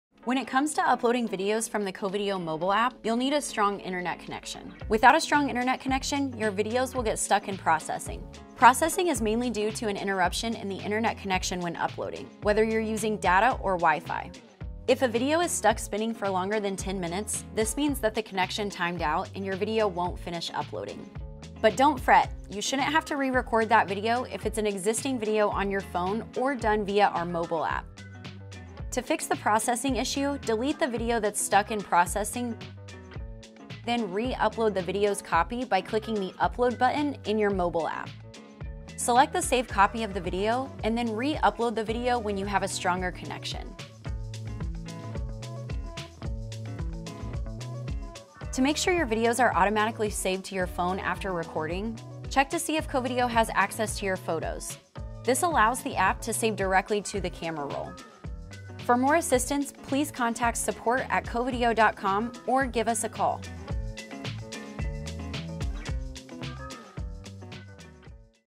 Covideo How To: Fix Your Processing Video on the Mobile App
Without a good internet connection, your videos can get stuck in processing. Watch the video to learn how to fix processing and how to avoid them in the future.